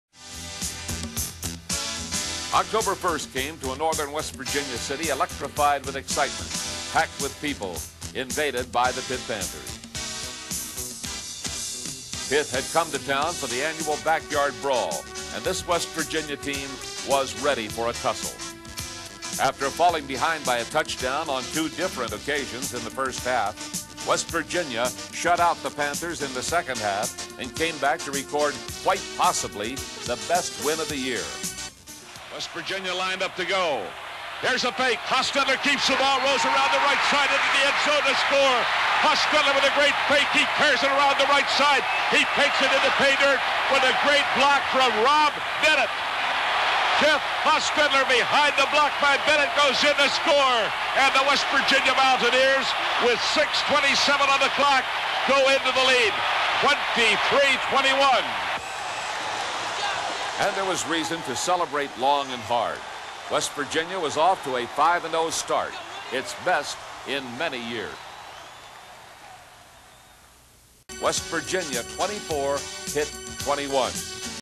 1983 Backyard Brawl
The 1983 Backyard Brawl featured future New York Giants Super Bowl winning quarterback Jeff Hostetler in his senior year at WVU.  The 2-1 Pitt Panthers came into 4-0 WVU's home turf for the annual matchup, and it was a beauty.  Pitt took a one touchdown lead twice in the game, but The Hoss kept WVU close, and the WVU defense held Pitt scoreless in the second half, setting the stage for the Mountaineers to take the lead late in the game.